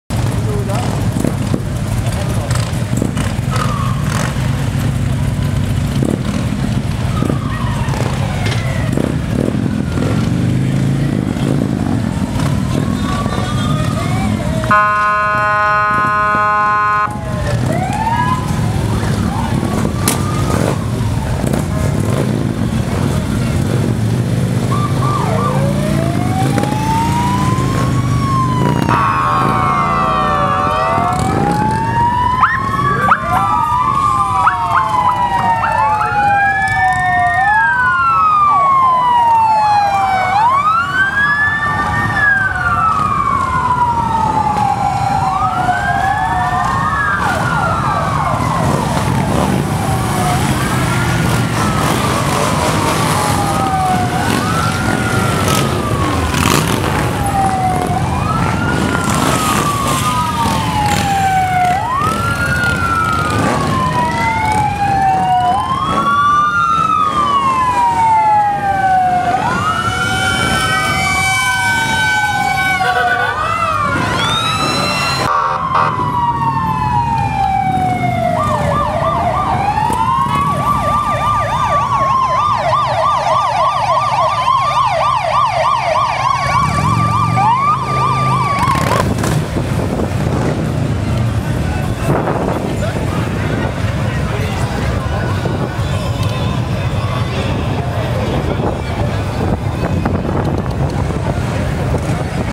Rassemblement Harley en Bourgogne du Sud
En ce week-end prolongé l'Alsace Chapter Club de Strasbourg a décidé de venir à rencontre des membres du club de Chalon-sur-Saône.
L'occasion de découvrir la Saône-et-Loire en particulier et la Bourgogne en général.